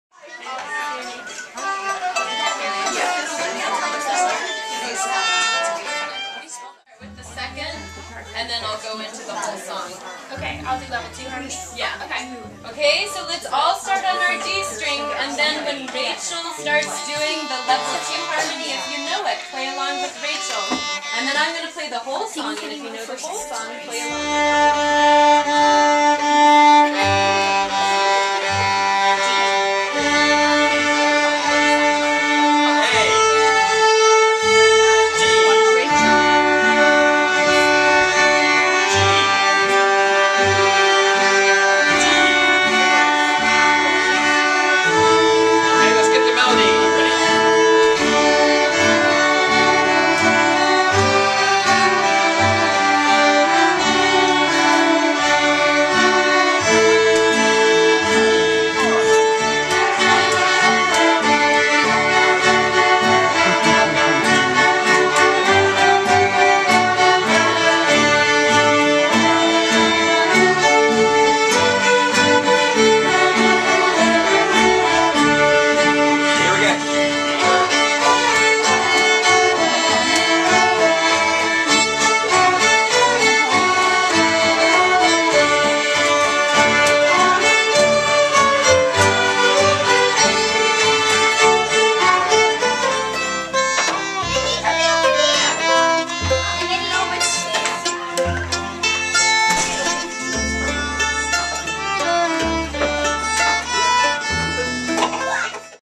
Creekside Strings Fiddle Jam November 2016
Here is a few images from a typical jam of Creekside Strings, a FiddleQuest studio in Ashland, Oregon. Nothing fancy, just lots of fun, food and laughter.